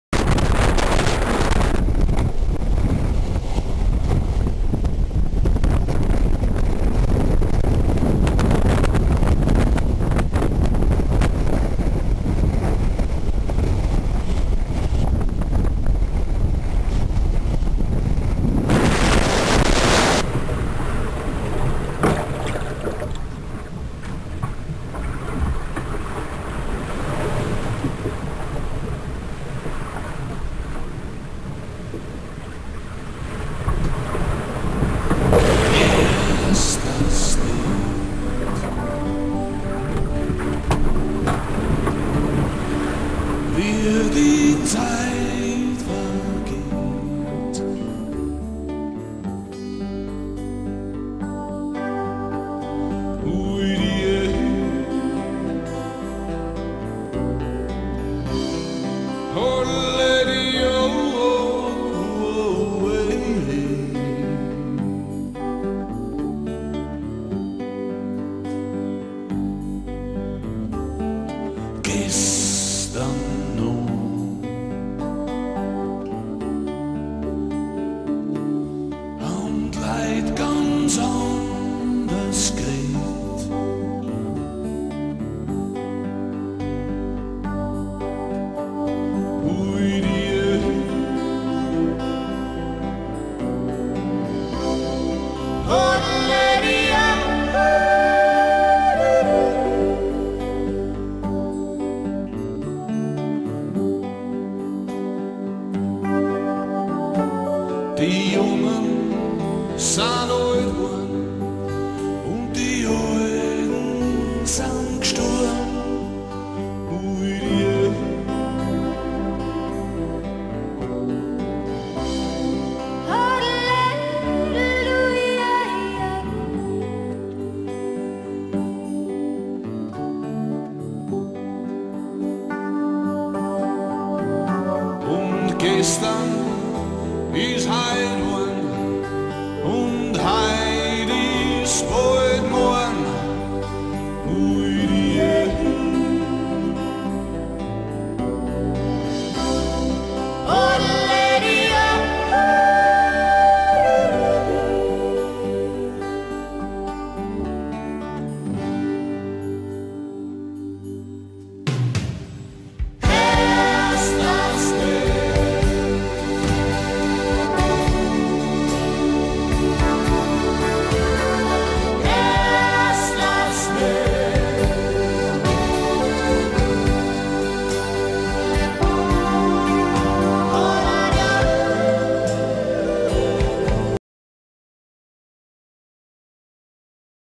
Chagos Archipel.wmv
In the middle of the Indic Ocean  - uninhabited islands somewhere between Maldives , Seychelles and Mauritius.
With little damage We have escaped a Kat. 2 Cyclone out of time, which spread his strength exactly over us. When we arrived in Salomon Island an Atoll of the Chagos Archipelago it was like Paradise - for three month.